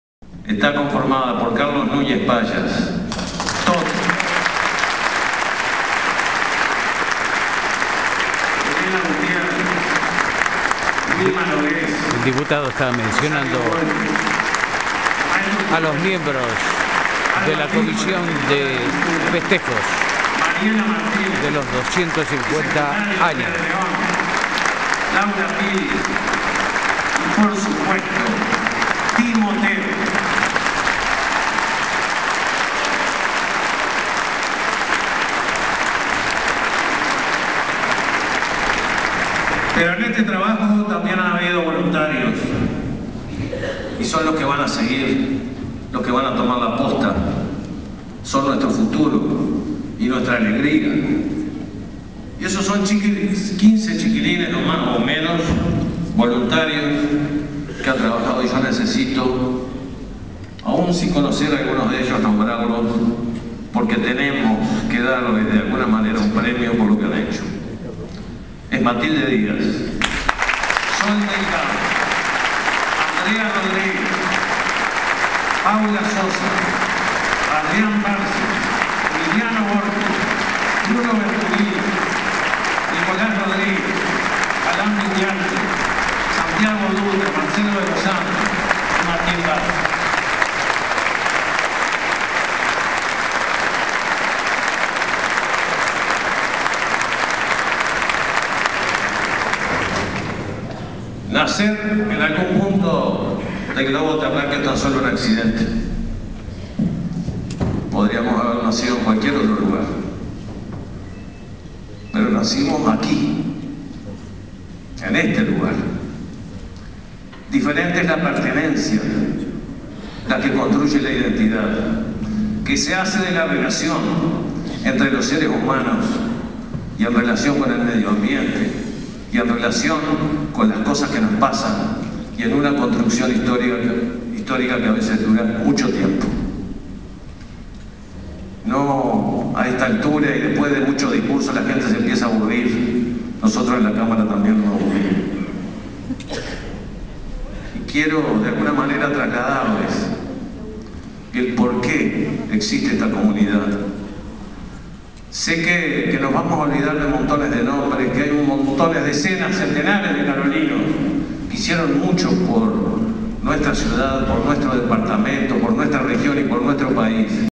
San Carlos Celebra sus 250 años - Sesión Solemne Cámara de Diputados - 14
La ciudad de San Carlos en el departamento de Maldonado en la República Oriental del Uruguay, celebró los 250 años de su fundación, siendo este histórico hecho acompañado, entre otras actividades, con una Sesión Solemne de la Cámara de Diputados.-    08-07-13